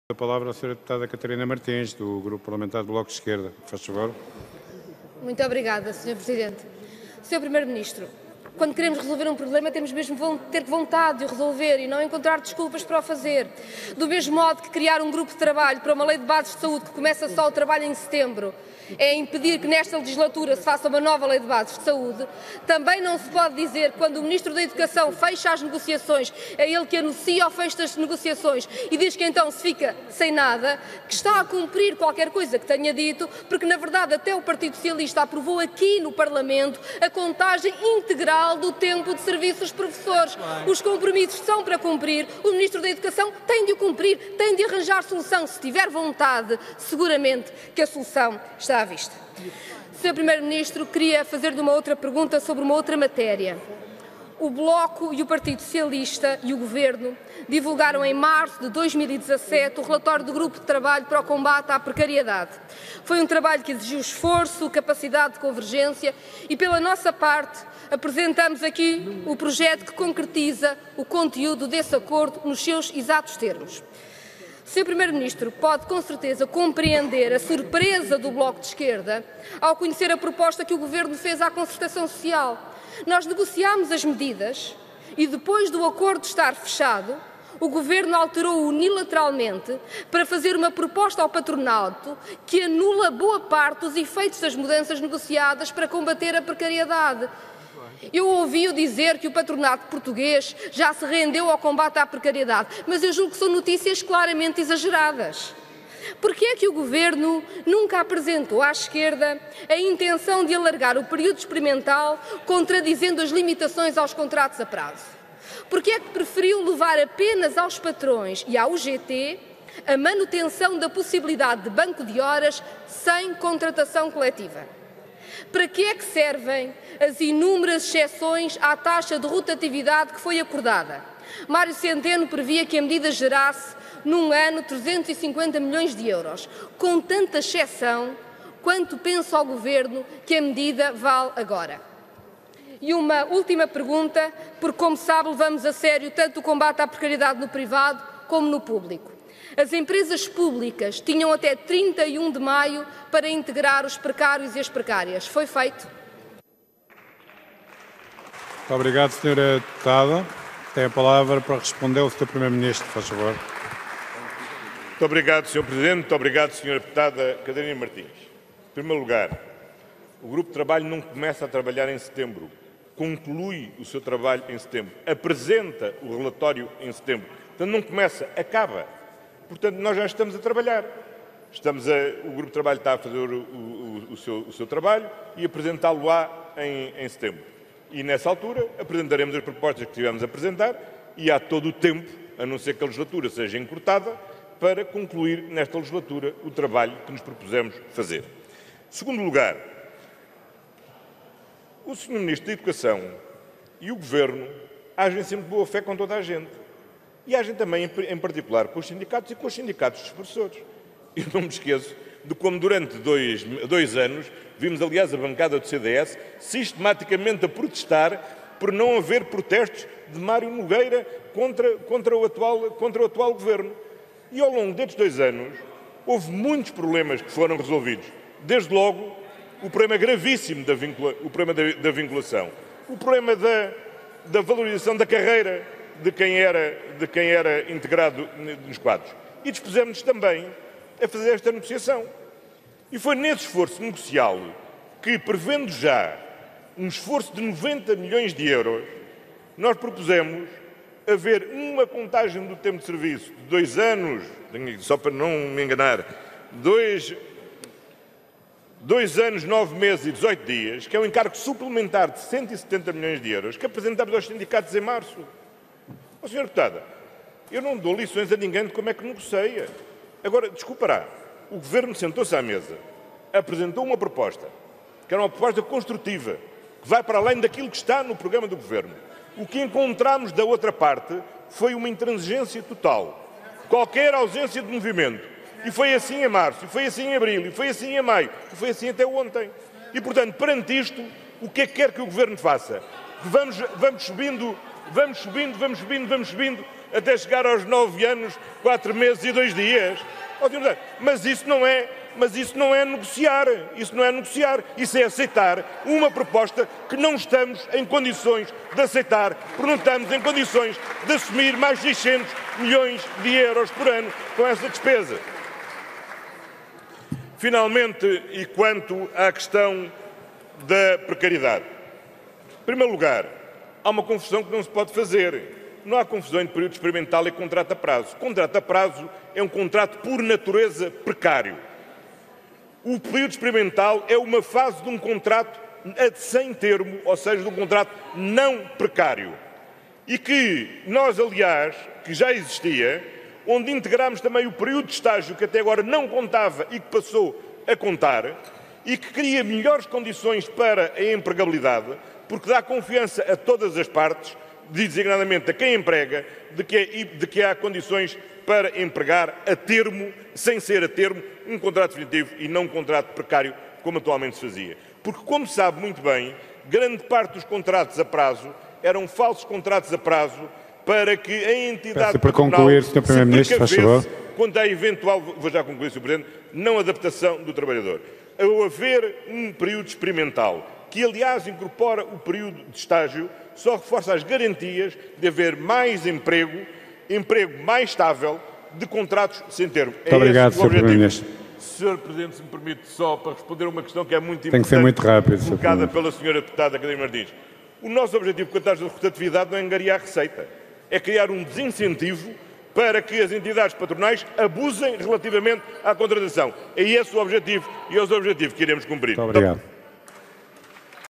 “Proposta do governo anula boa parte do efeito das mudanças negociadas para combater à precariedade”
Na terceira parte da sua intervenção no debate quinzenal com o primeiro-ministro, Catarina Martins insistiu para que o governo cumpra o acordo com os professores e encontre soluções para o tempo de contagem da carreira dos professores. Voltou a criticar o recente acordo de concertação social entre o governo, o patronato e a UGT,  que no essencial signfica o incumprimento do acordo estabelcido com entre o governo e o próprio Bloco de Esquerda| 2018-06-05